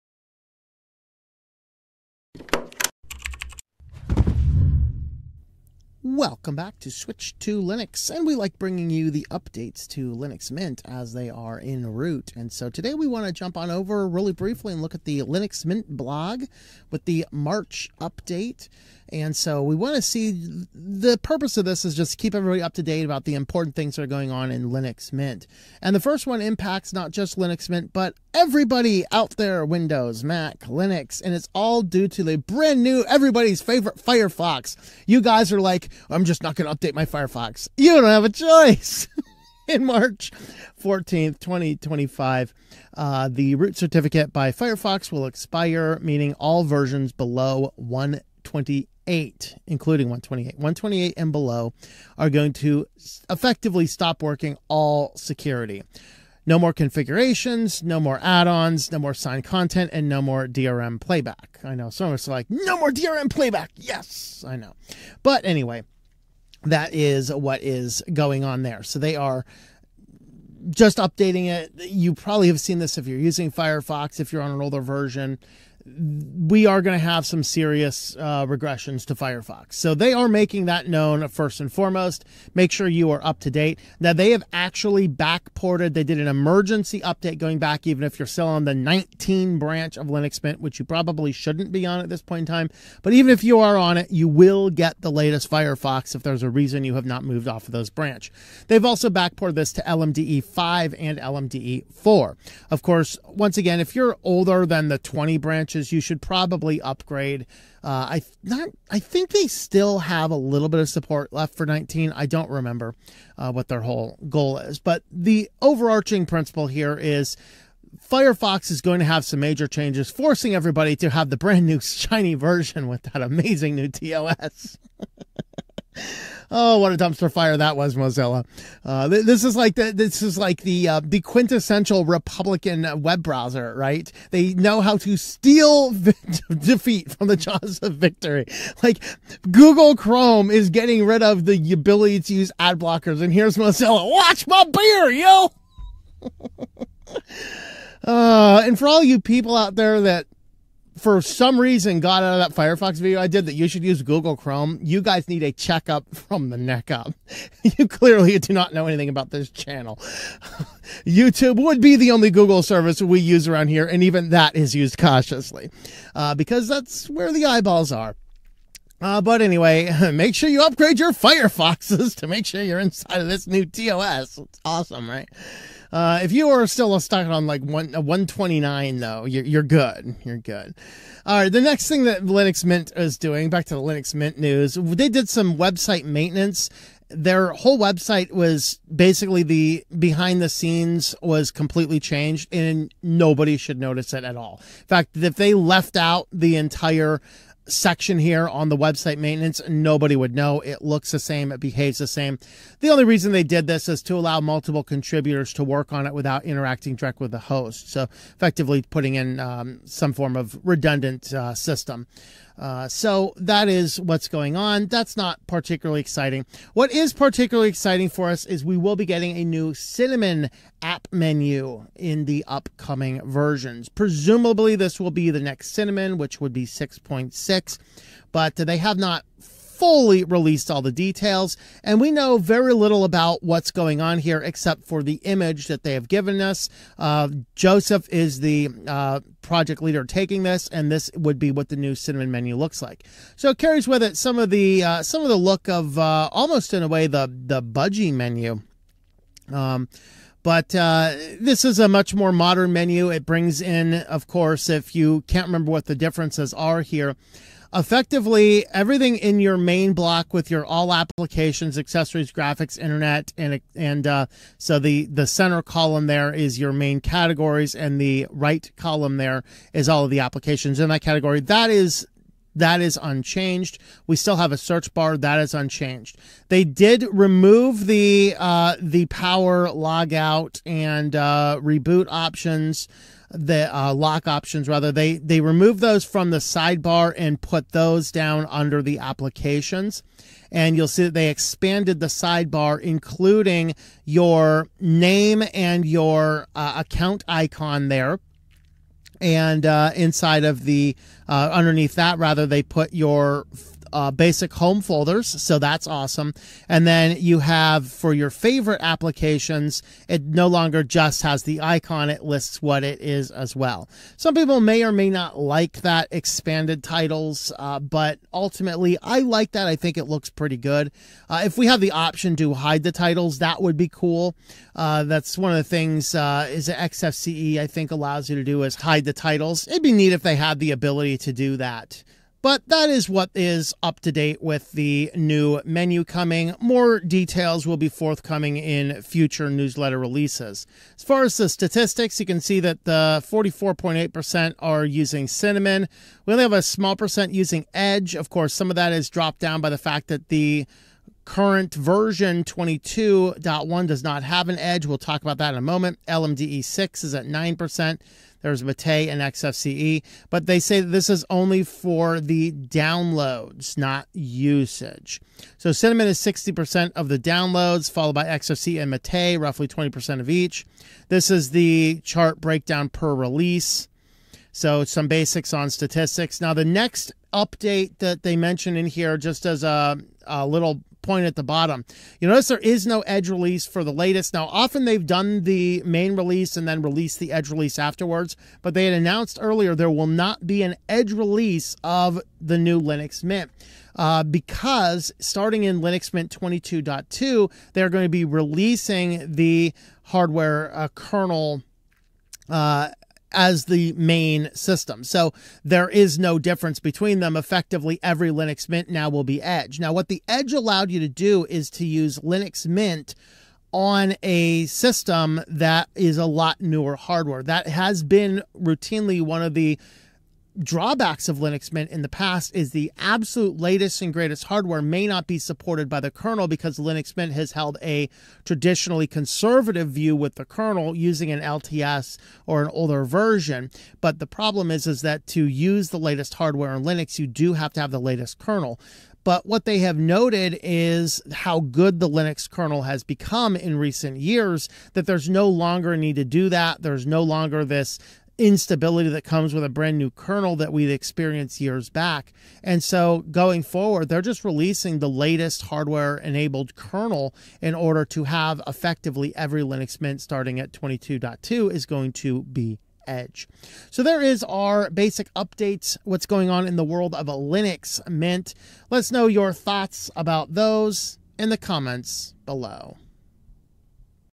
Linux Mint's New Cinnamon Menu
Today we look at the March news update for Linux Mint and what is upcoming to future Cinnamon Desktop releases.

Support Switched to Linux!
🐸 Gab: @switchedtolinux
💡 Minds: @switchedtolinux